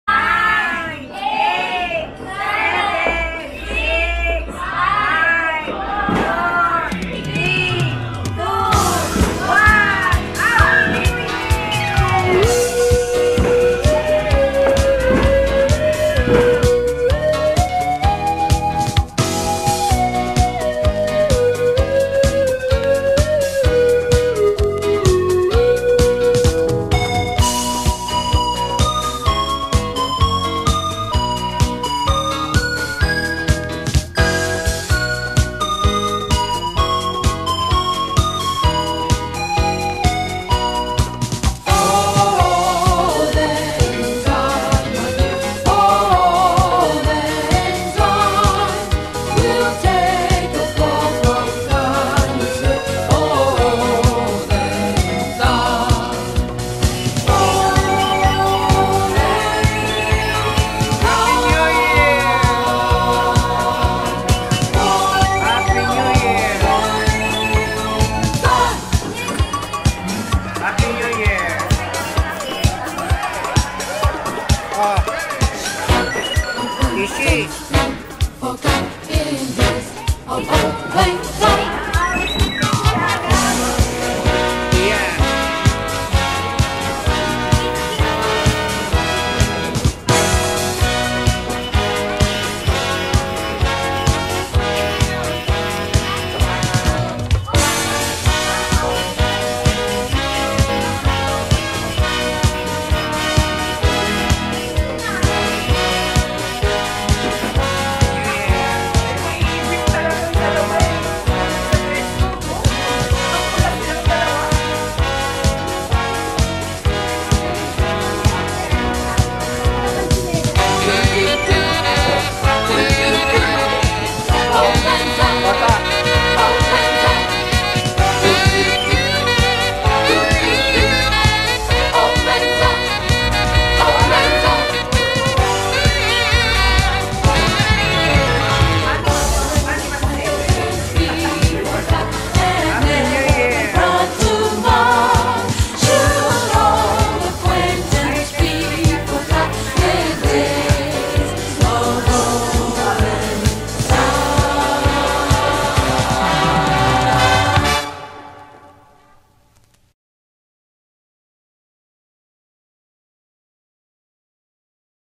My Family's 2021 New Year Celebration
We wanted to share with you our simple, and memorable 2021 New Year Celebration at home.  From our family to yours, we wish everybody, "A safer and healthier year for all of us, God bless!".